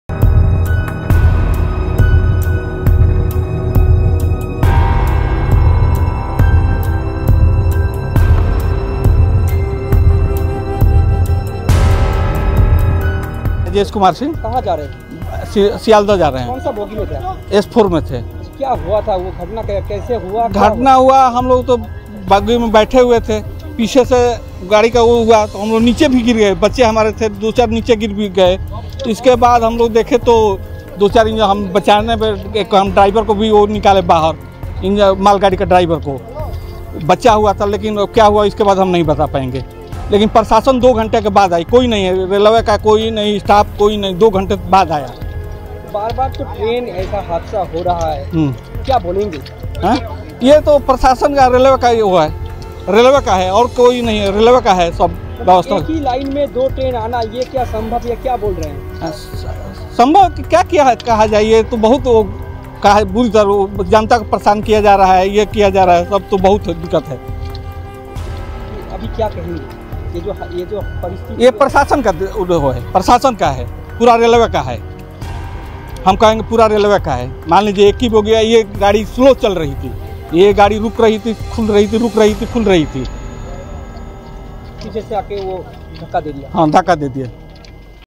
Darjeeling Train Accident: दार्जिलिंग रेल हादसे के चश्मदीद ने सुनाया खौफनाक मंजर, कैसी पलटी ट्रेन...
बंगाल के दार्जिलिंग में कंचनजंगा रेल हादसे का शिकार हो गई। एक ही ट्रैक पर दोनों गाड़ियों के आ जाने से भीषण हादसा हो गया। इस हादसे की तस्वीरें बहुत खौफनाक हैं। मौके पर राहत-बचाव कार्य जारी है। ऐसे में मौके पर मौजूद चश्मदीद ने क्या बताया सुनिए...